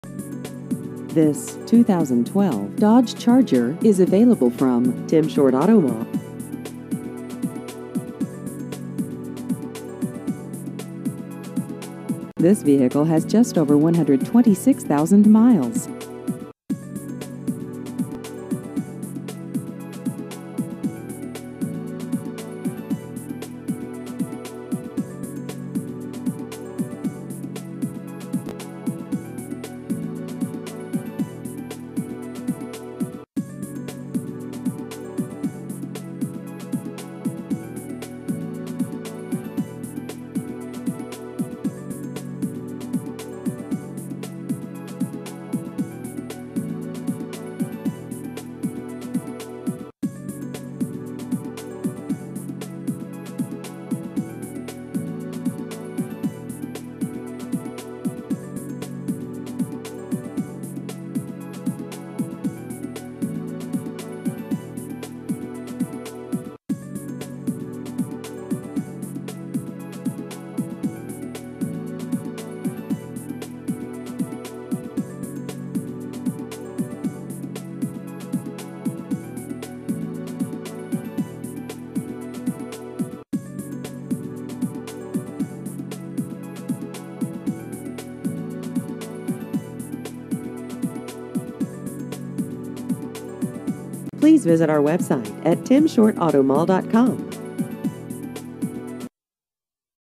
2012 Dodge Charger Used Cars Corbin KY
This 2012 Dodge Charger is available from Tim Short Auto Mall.